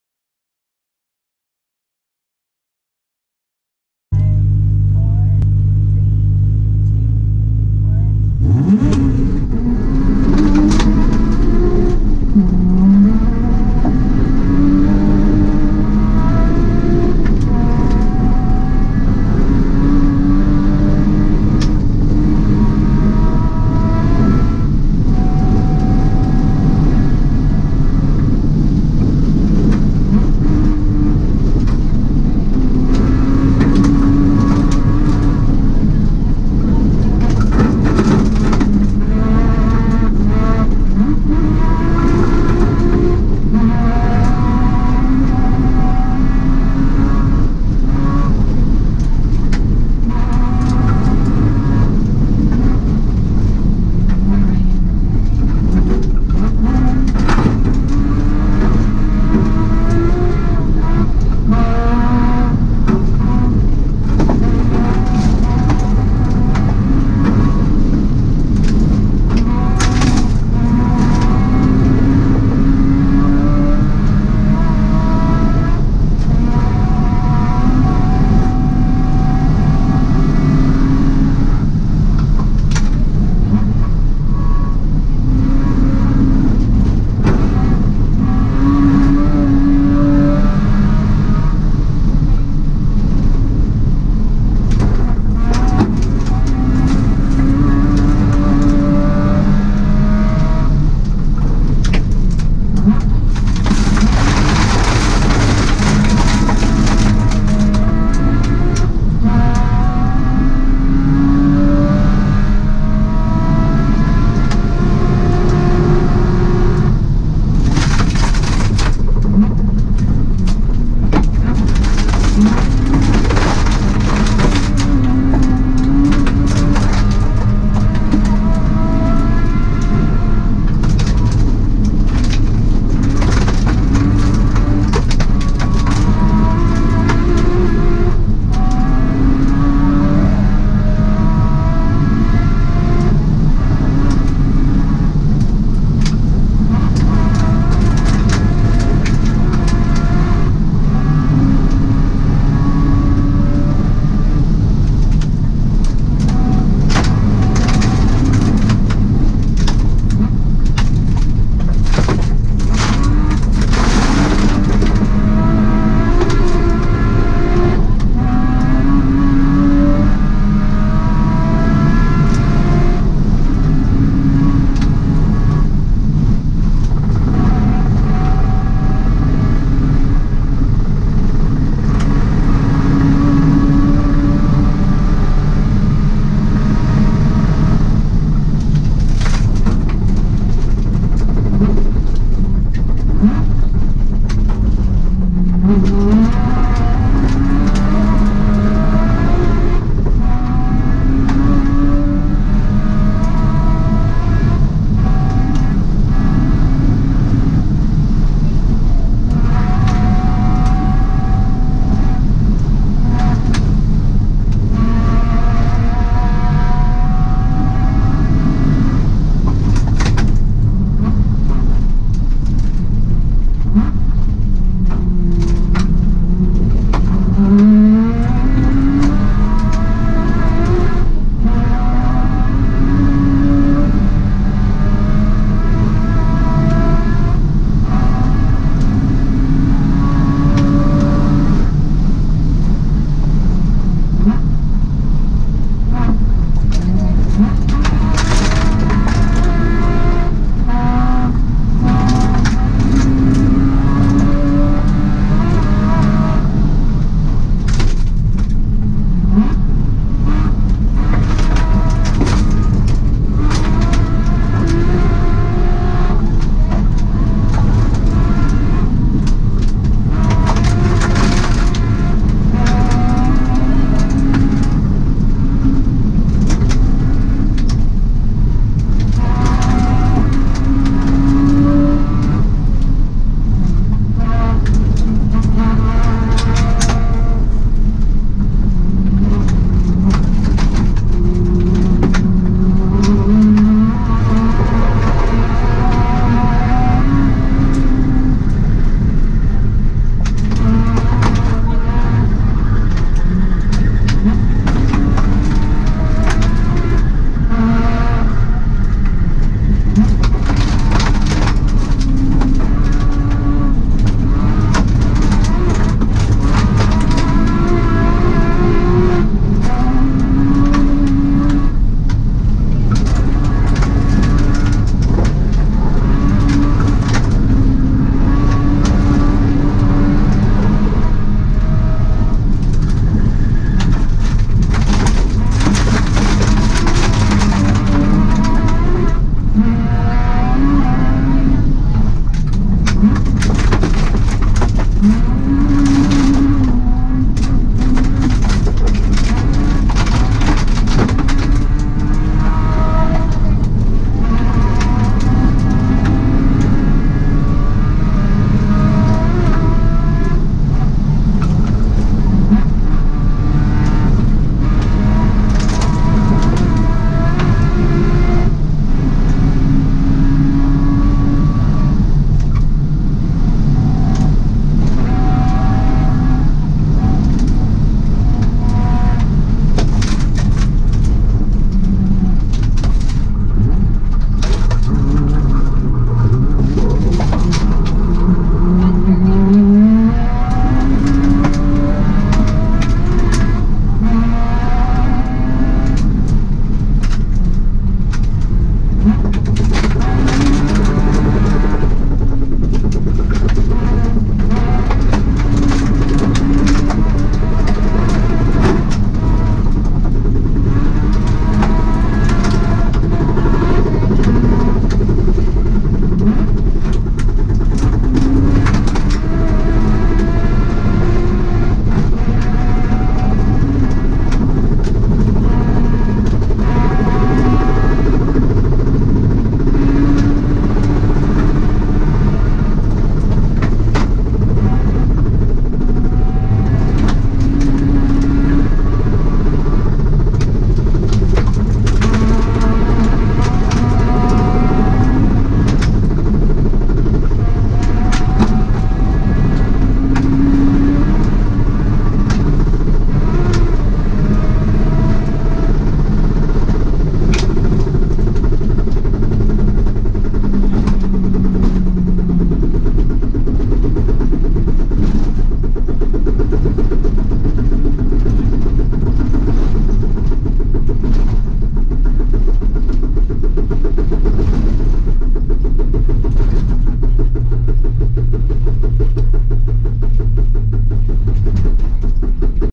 Sno*Drift Summer Rally 2019 SS8: Huff - Old State 2
It's not raining on this stage, I was using the wipers to get coolant off the windshield.  There was a hose clamp on a new hose that needed to be retightened.  I backed off a little at the end of the stage when the red light (mostly obscured by the steering wheel) came on.  That was the warning for oil temperature which I have set to turn on at 250º.